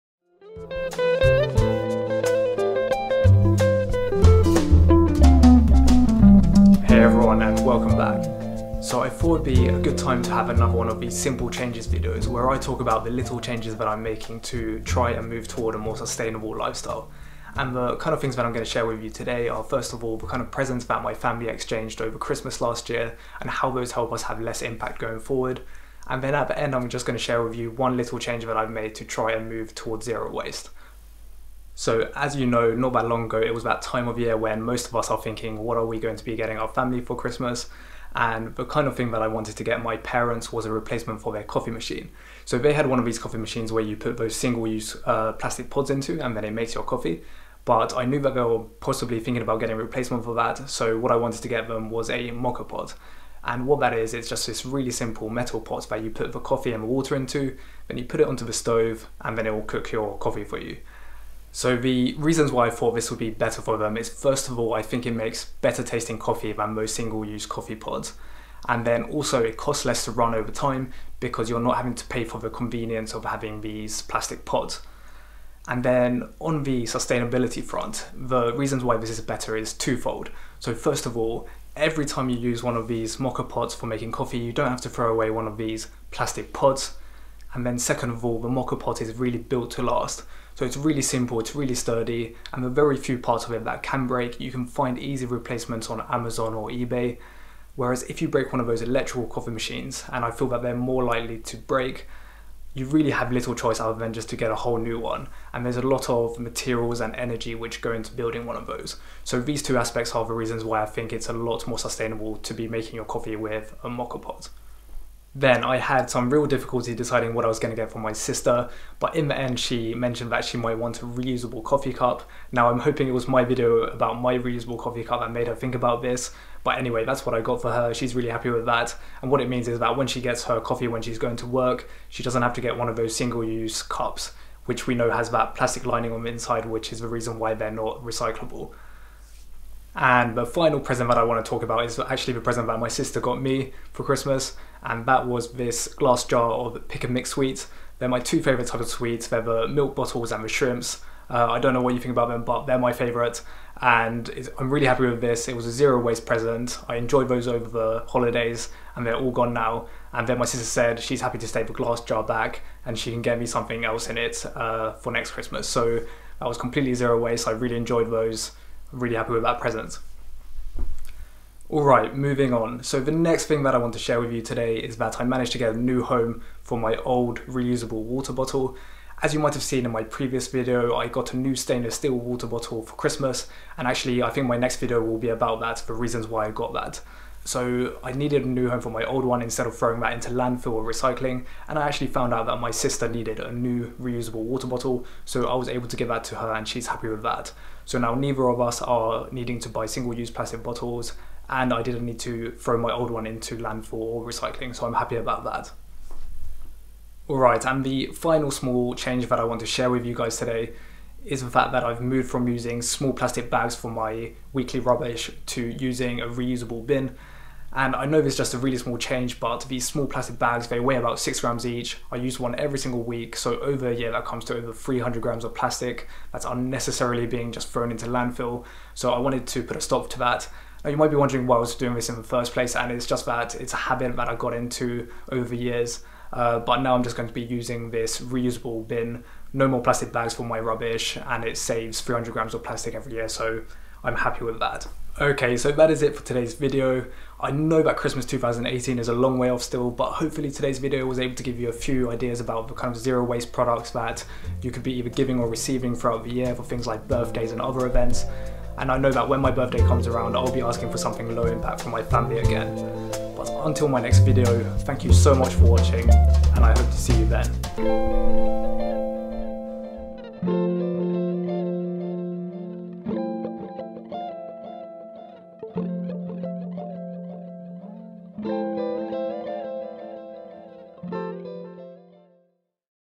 Zero waste coffee, gifts and more - Simple Changes #3
Today I’m sharing with you another handful of small efforts towards less waste and less impact. I tried to give and receive as many zero waste presents as possible last xmas; here are some of them.